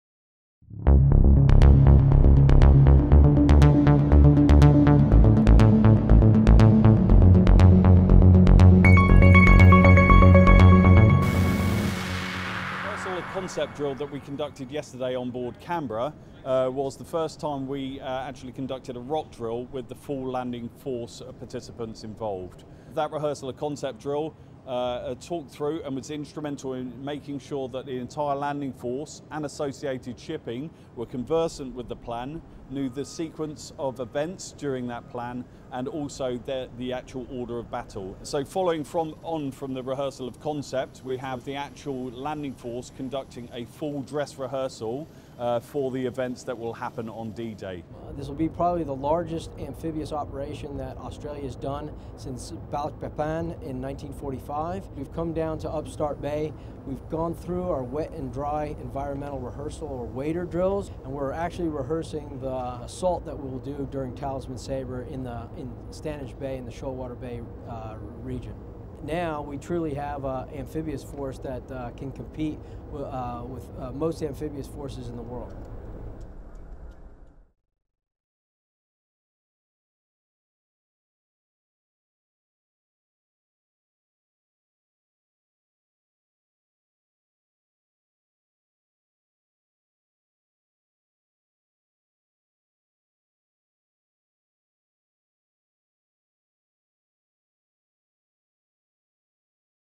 Talisman Sabre 2017 – HMAS Canberra landing rehearsal
HMAS Canberra has conducted amphibious landing rehearsals ahead of the main beach landing at Exercise Talisman Sabre.

The landing rehearsals at Upstart Bay in central Queensland aimed to iron out any kinks or cobwebs ahead of the what will be a major and very complex beach assault during the exercise proper.

Defence says the beach assault during Exercise Talisman Sabre will be the ADF's largest such insertion since World War II.

Exercise Talisman Sabre 2017 (TS17) is a biennial combined Australian and United States training activity, designed to train respective military forces in planning and conducting combined task-force operations to improve the combat readiness and interoperability between the respective forces.

This exercise is a major undertaking that reflects the closeness of the two alliance and the strength of the ongoing military-to-military relationship.

The exercise involves more than 30,000 soldier, sailors, airmen and marines.

Now in its seventh iteration, the Talisman Sabre series of exercises is the principal Australian and US military training activity focused on the planning and conduct of mid-intensity high-end warfighting in the air, land and maritime domains.

TS17 incorporates force preparation activities, special-forces activities, amphibious landings, parachuting, land-force manoeuvre, urban operations, air operations, maritime operations and a number of live-fire components.

Sincerely,

Brian Hartigan
Managing Editor

Minnamurra
NSW 2533
AUSTRALIA